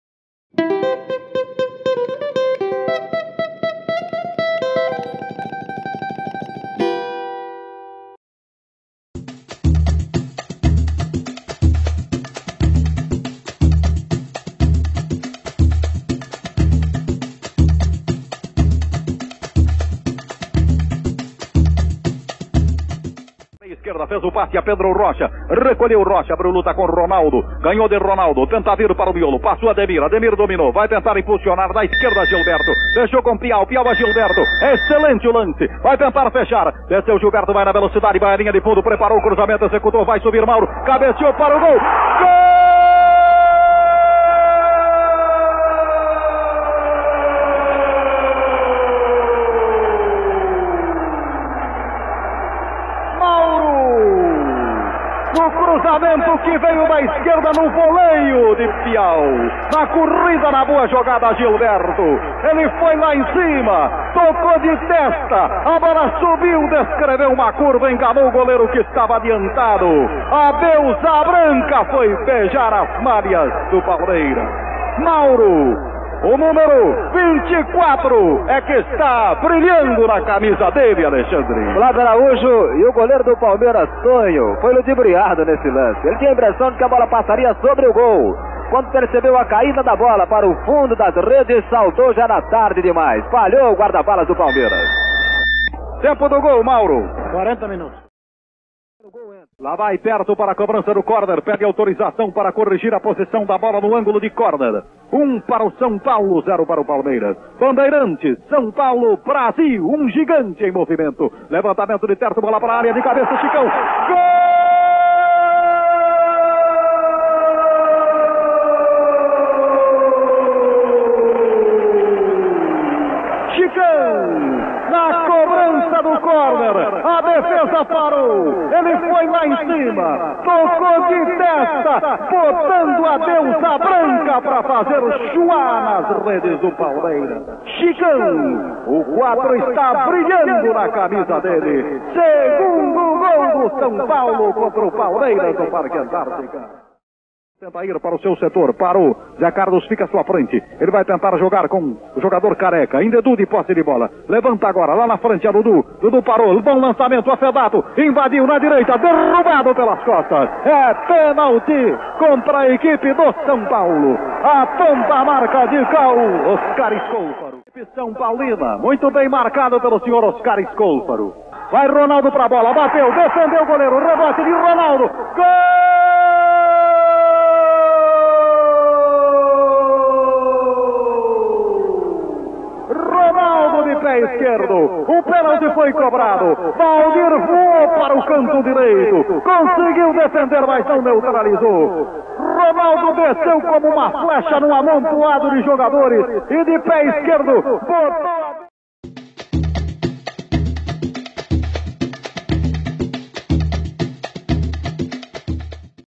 Palmeiras 1x2 São Paulo - 1974 - TRICOLOR VENCE O CHOQUE REI PELA LIBERTADORES NA CASA DO RIVAL!
Dividindo o grupo da primeira fase com Palmeiras, Deportivo Municipal e Jorge Wilsterman, o tricolor do técnico José Poy foi ao Parque Antártica e com imposição de seu estilo, venceu o rival pela segunda vez e encaminhou a classificação, que culminaria na grande campanha de vice campeonato.

MORUMBITECA
O maior acervo de vídeos do São Paulo FC